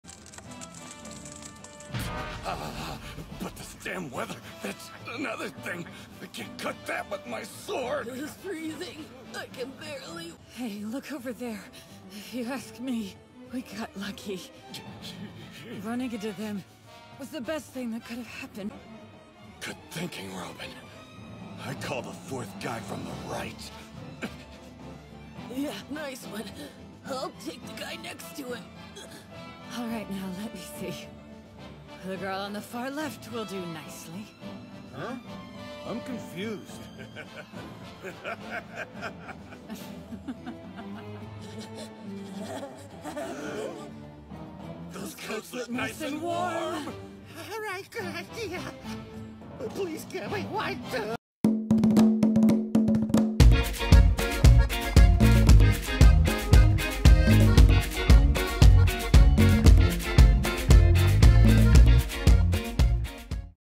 Straw hat's evil side - rampage
straw hat's stealing coats from other's